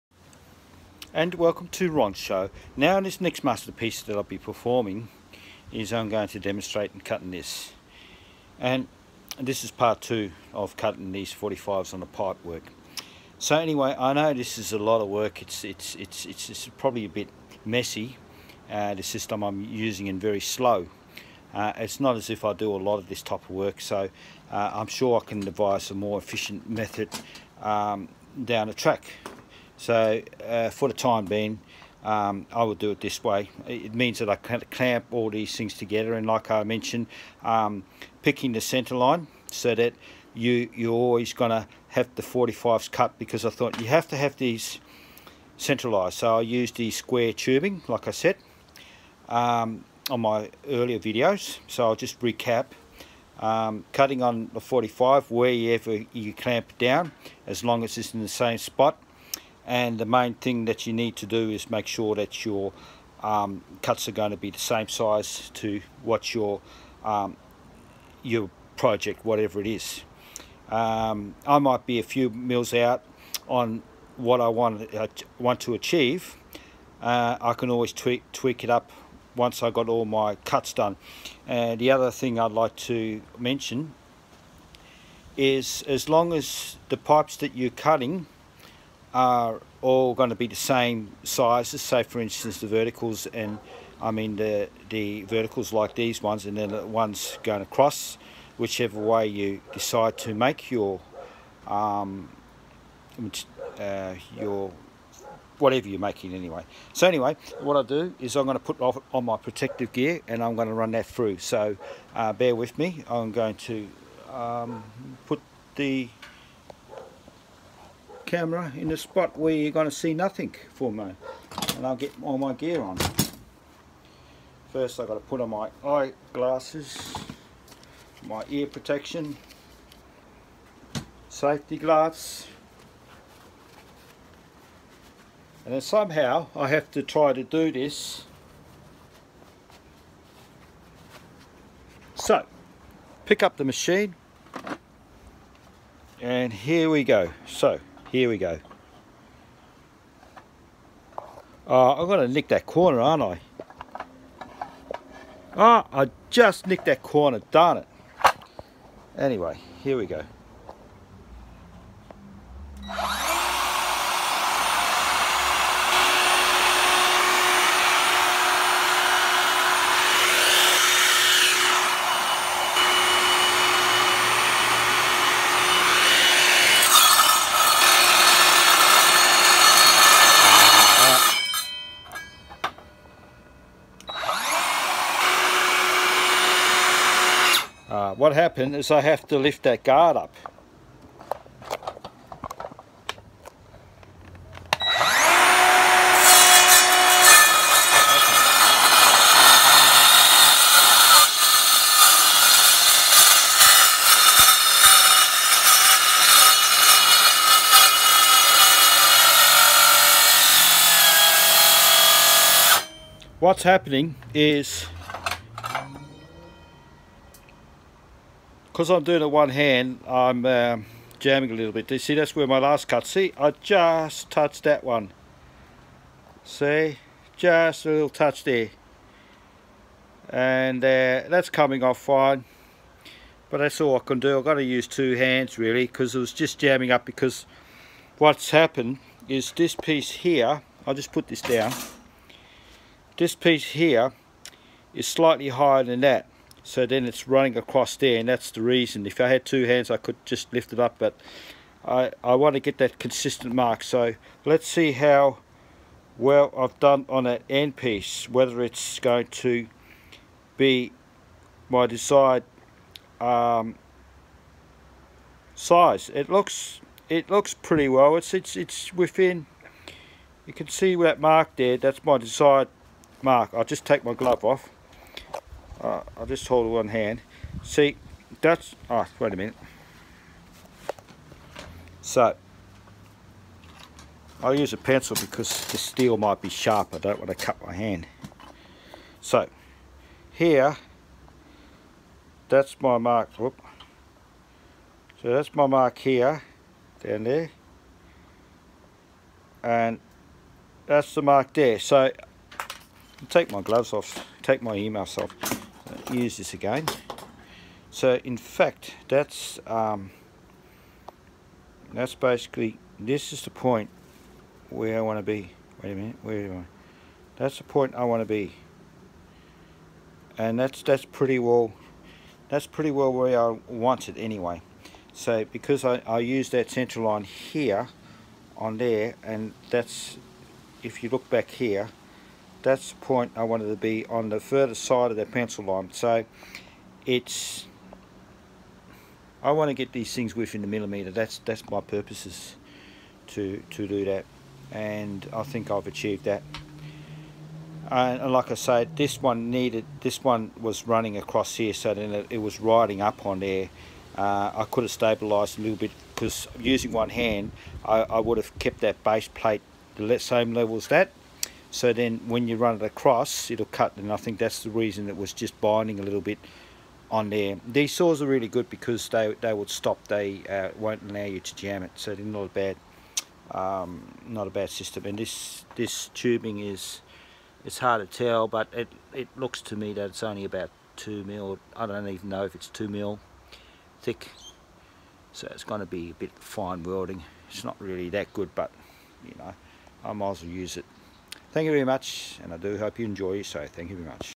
2 November 2020
Cold cut saw on pipe at 45 degree cordless part 2. The cut.